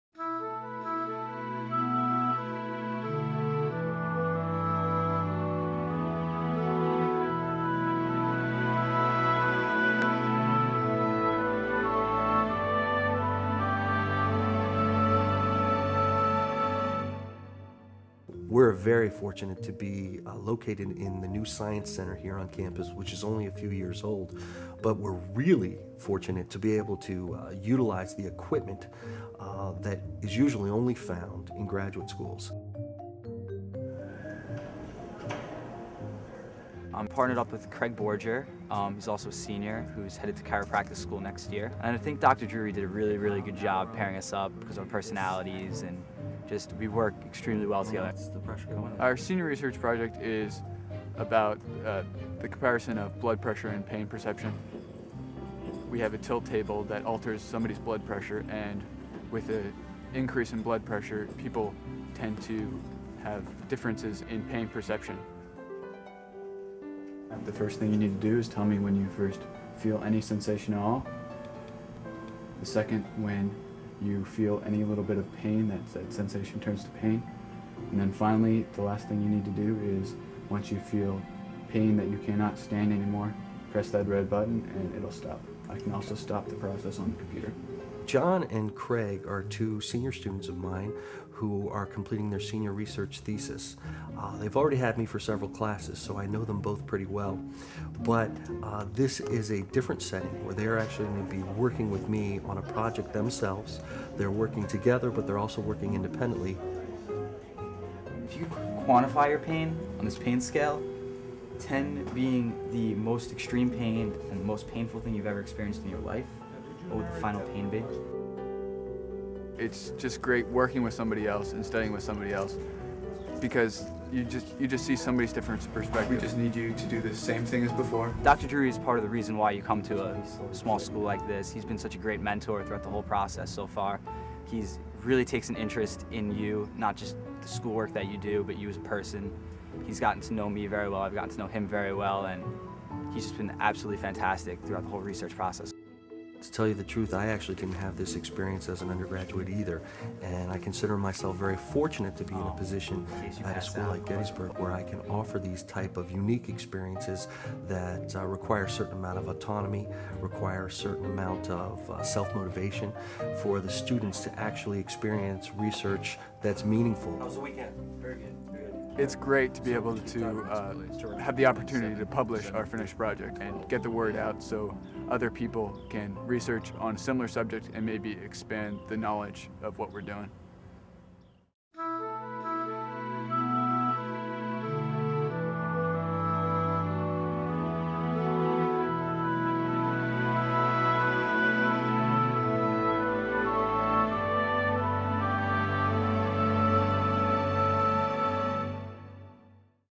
Gettysburg College - Undergraduate Research - Health Sciences
Craig Borger and Jon Giordano are doing the kind of independent research that-while standard for a Gettysburg senior's capstone experience-is more characteristic of a graduate-school program. But here, a committed faculty and cutting-edge resources combine for an experience that enhances the learning process in extraordinary ways.